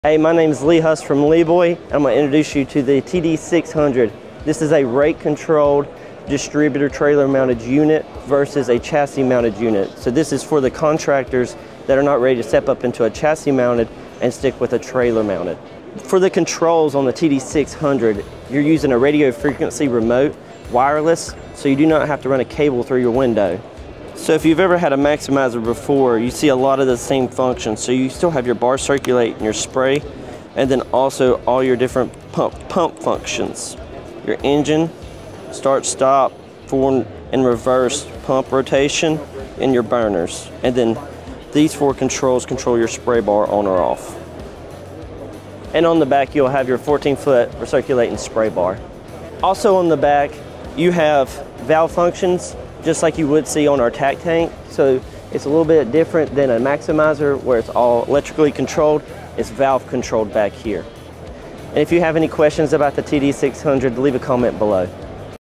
TD600 Tack Distributor is what you need!
Introducing the all new TD600 Tack Distributor, stop by booth #2301 to be the first to see this new product.